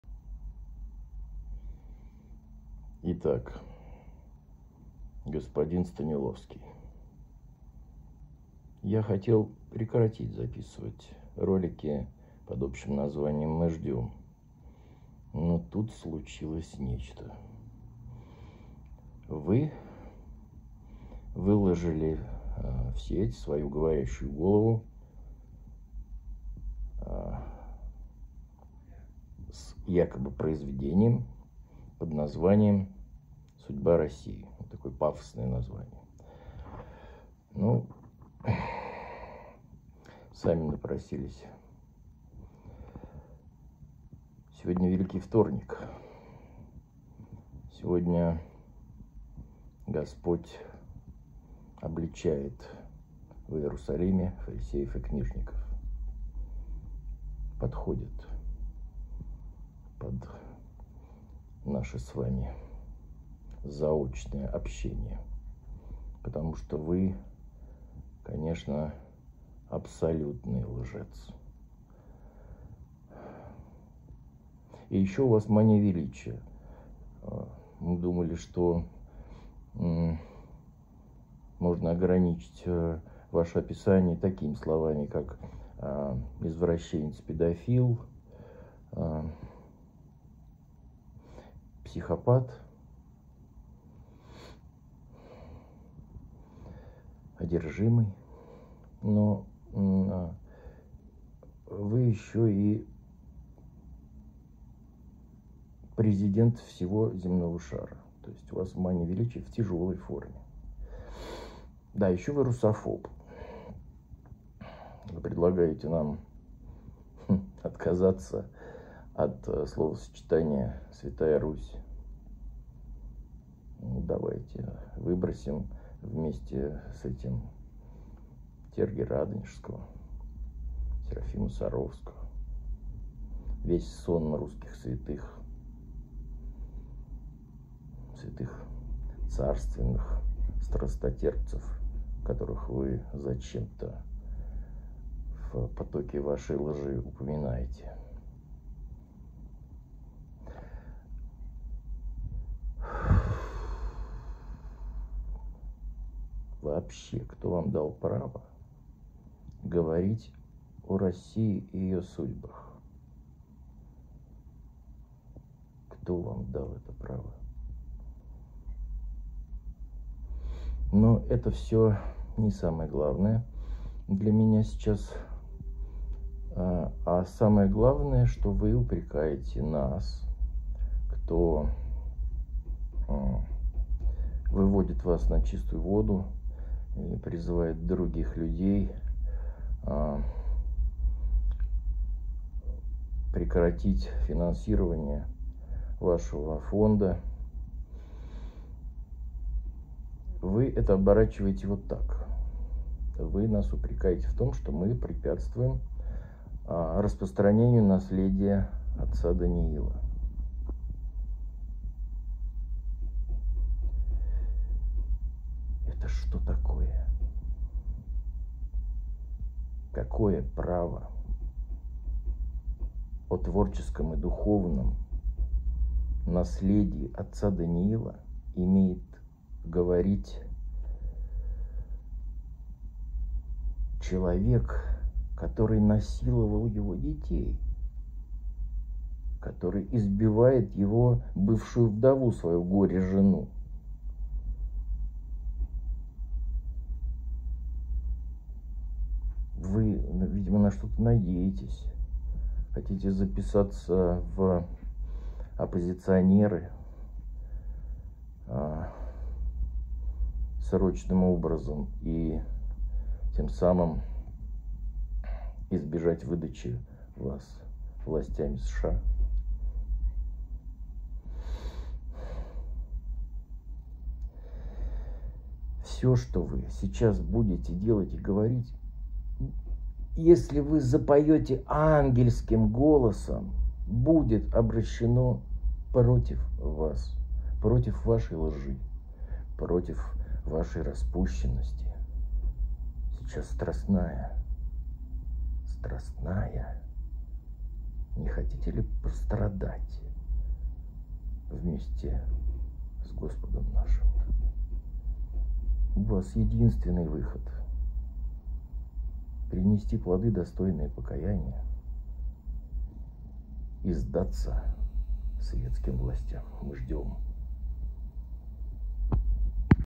Мы ждем. Великий вторник.
Обращение к Сергею Станиловскому - мучителю детей о. Даниила Сысоева в связи с выходом его "фильма" - "Судьба России".